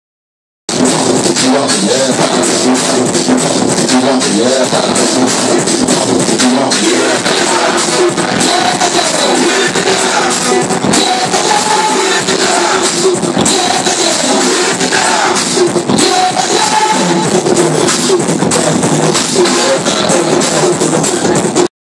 Dj Che Mixx n Dj Breezy D @ Rayburn homecomibg
Dj Che Mixx n Dj Breezy D(REVOLUCION DJz) @ Rayburn's homecoming! Everyone dancing the wobble